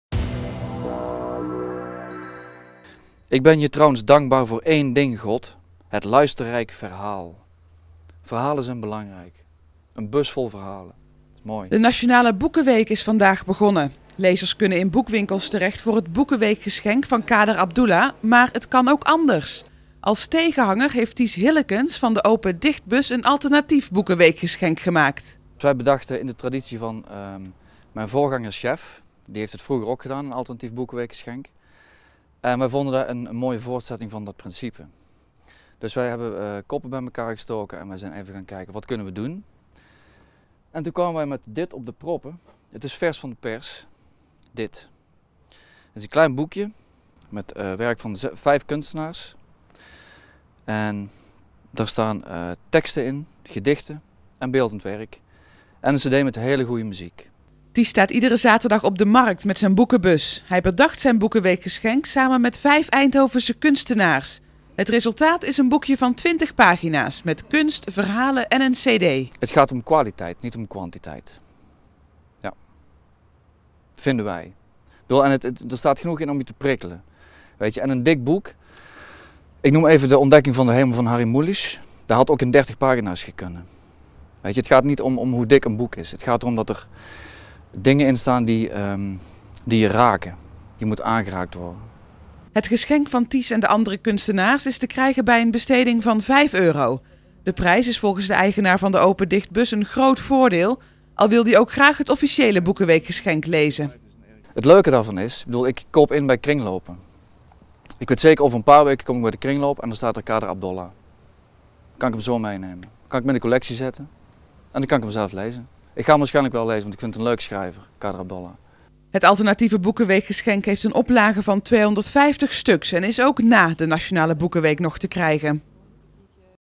Eindhovens boekenweekgeschenk
Ties Hillekens geeft een Eindhovens boekenweekgeschenk uit.